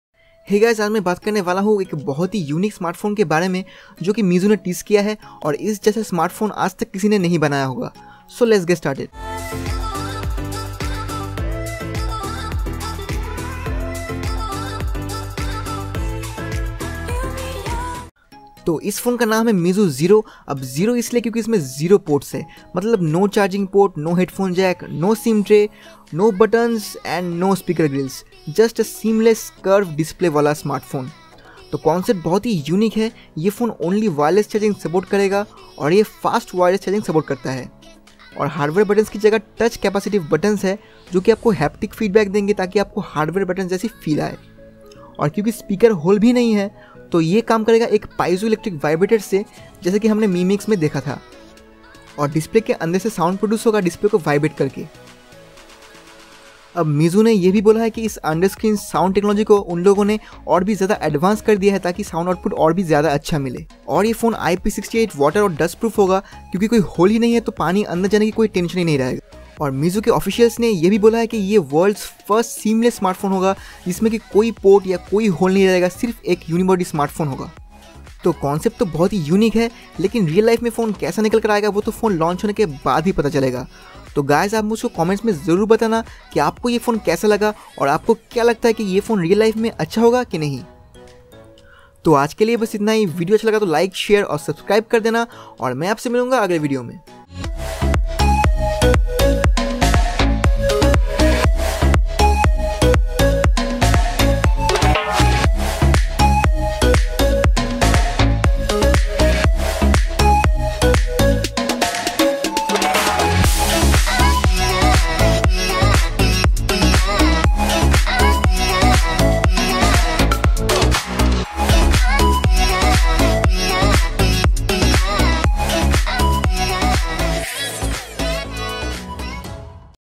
Meizu Zero OFFICIAL Hands On 2019 | Meizu Zero Trailer
So here is MEIZU ZERO Complete RENDERS/LEAKED IMAGES along with OFFICIAL TEASER!